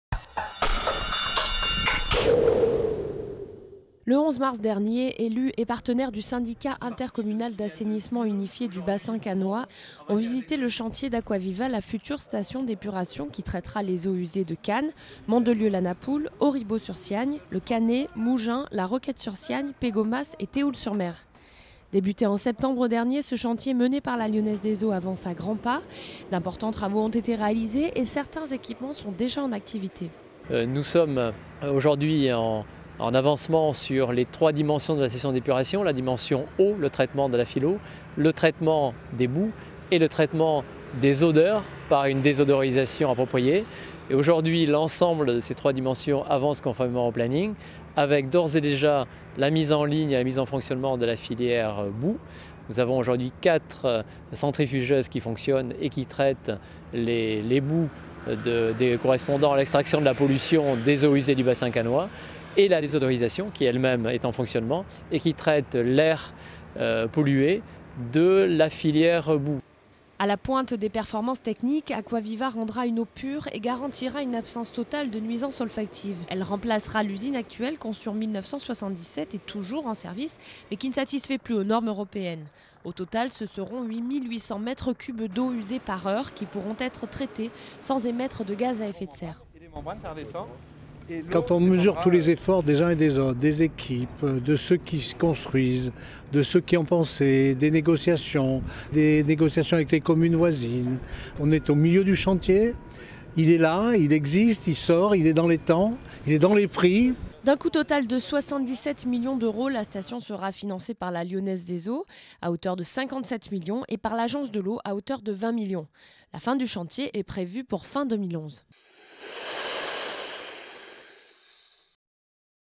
Visite chantier station d'épuration Aquaviva
Visite du chantier de la nouvelle station d'épuration du bassin cannois - (11/03/2010)